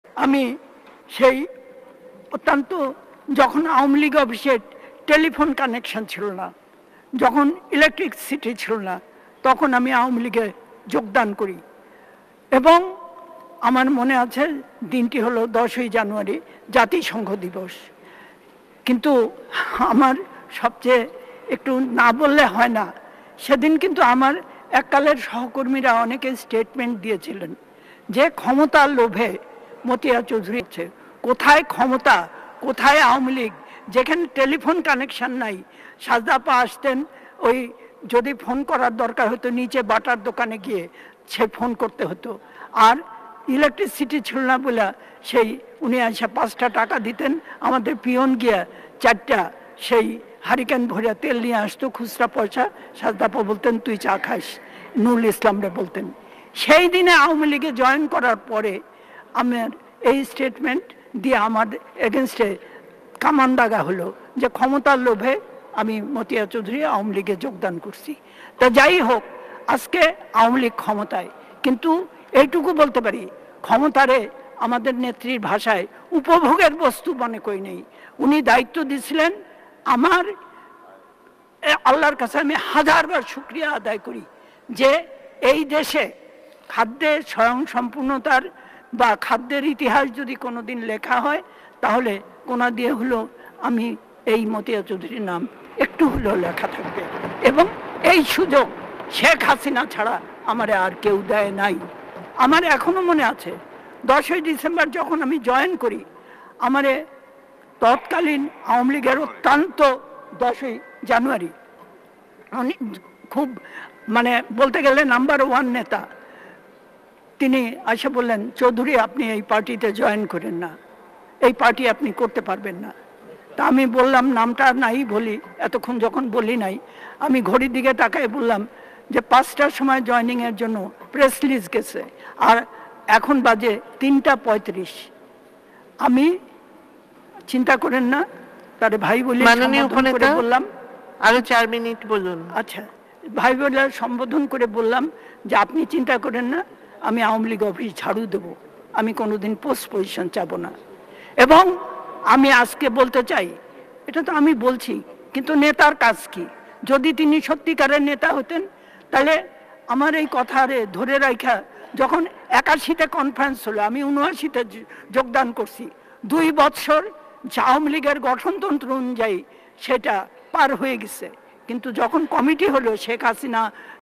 মতিয়া চৌধুরীর আওয়ামীলীগে যোগদানের ইতিহাস শুনুন | বেগম মতিয়া চৌধুরী
মতিয়া চৌধুরীর আওয়ামীলীগে যোগদানের ইতিহাস শুনুন | আওয়ামী লীগের প্রেসিডিয়াম সদস্য মতিয়া চৌধুরী মারা গেছেন | আওয়ামীলীগের যোগ দেয়ার ইতিহাস জানালেন মতিয়া চৌধুরী